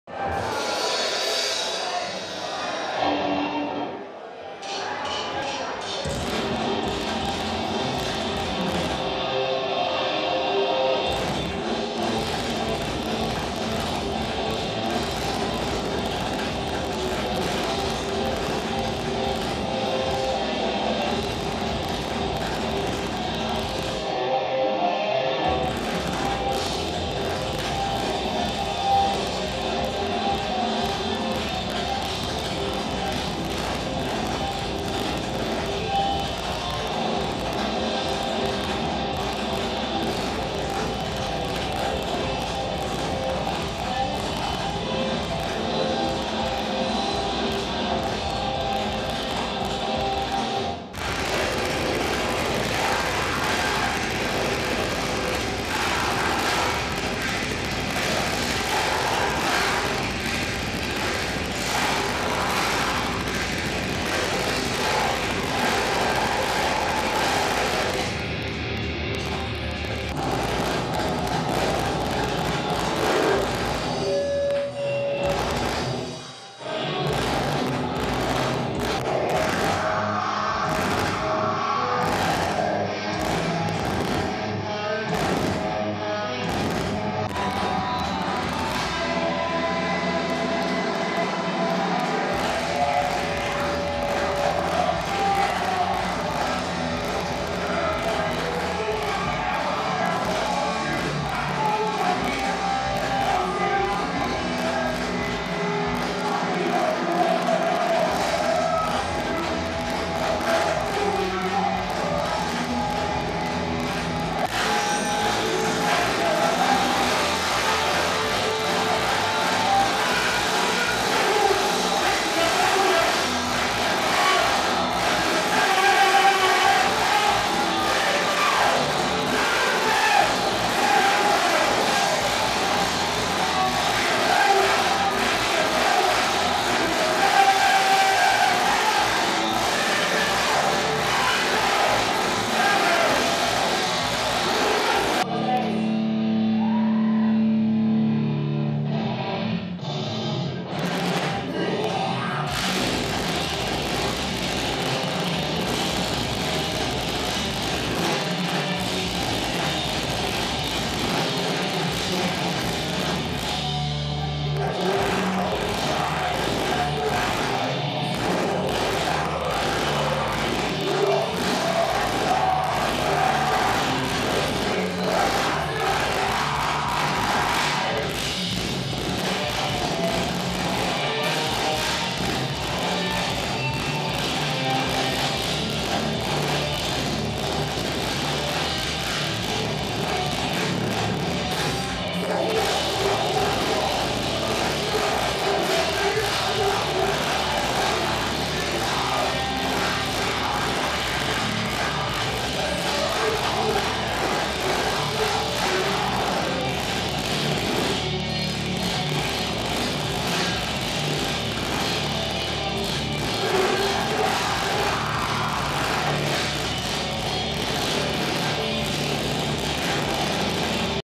South African Metal
South African metal bands performing at the Rumours Lounge in Johannesburg.